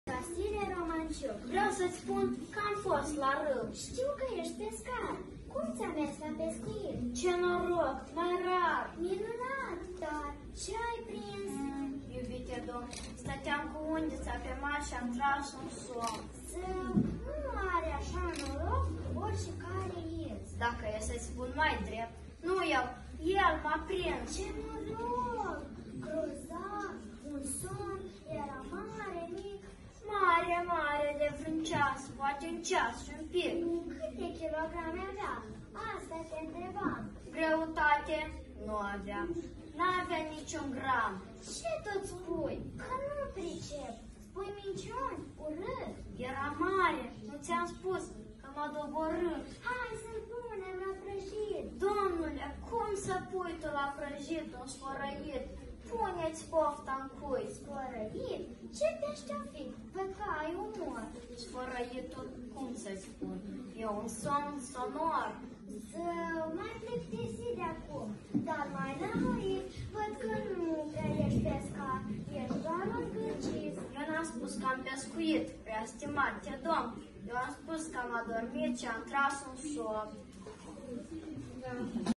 Chişinăul citeşte 2022
Veceslav Dudnic şi Ariana Portarescu au prezentat o frumoasă înscenare a poeziei  "Somnul lui Păcală" scrisă de V. Romanciuc, la conferinţa de totalizare în baza cărţii "Toate întâmplările se prefac în cuvinte" scrisă de Vasile Romanciuc, segmentul copii. Bravo!!!